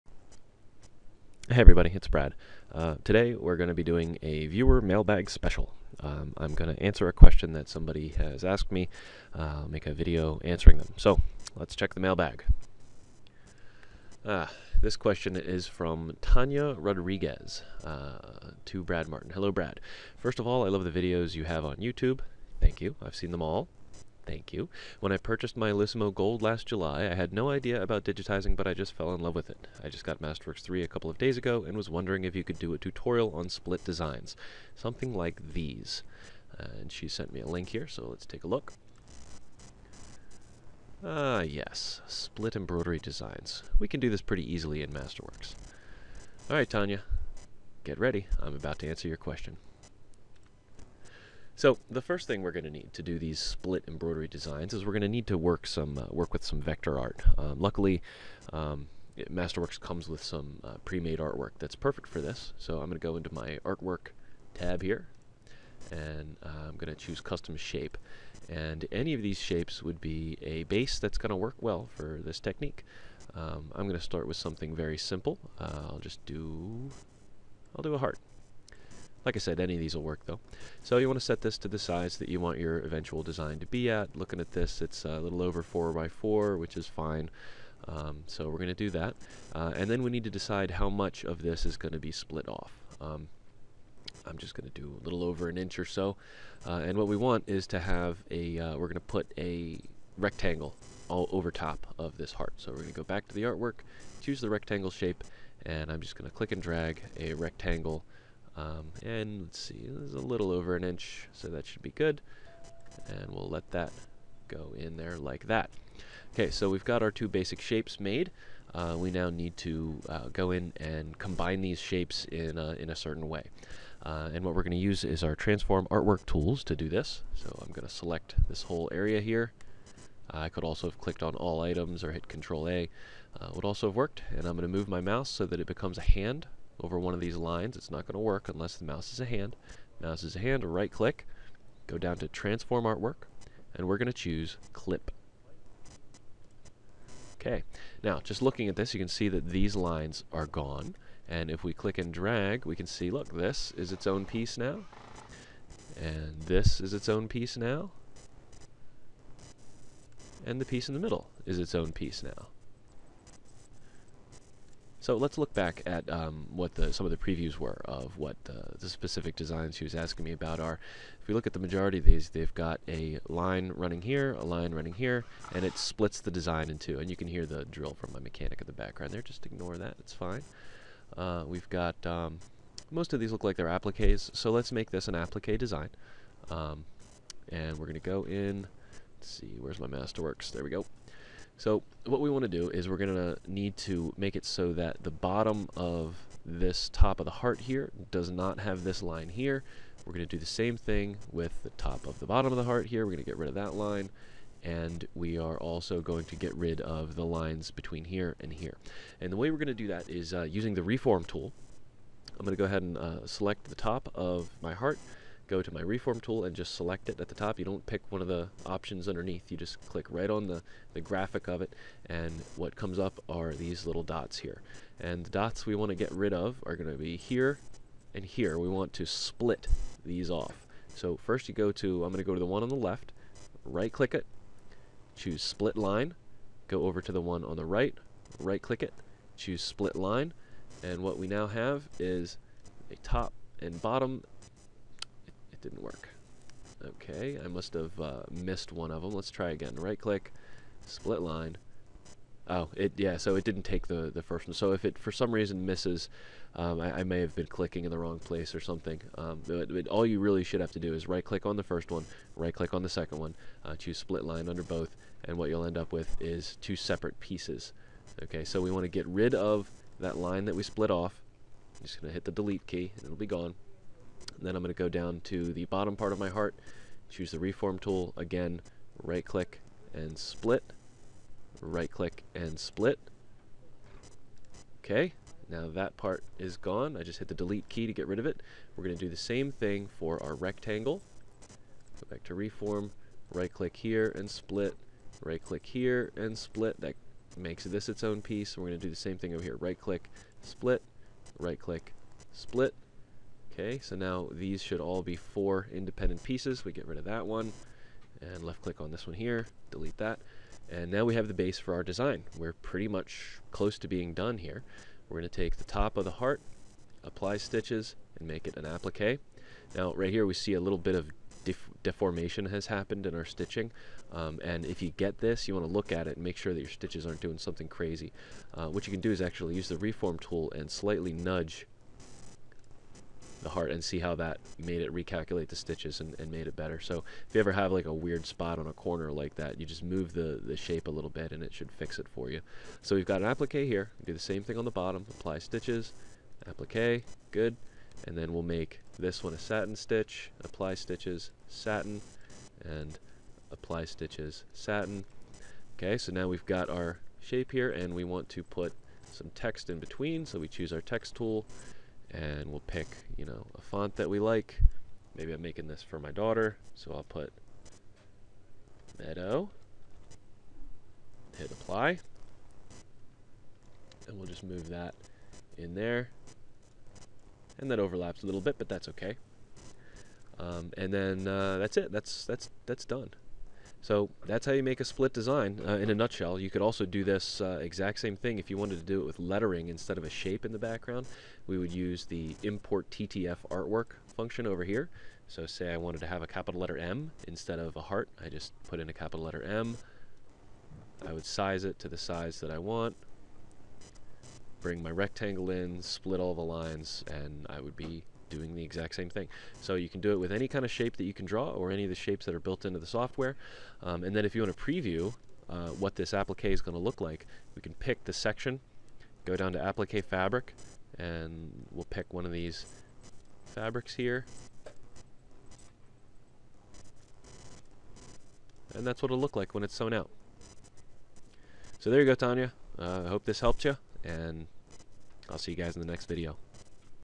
MasterWorks III - Viewer Mailbag Special - Split Embroidery Designs
In this video I answer Tanya's question about how to do a split embroidery design in MasterWorks III.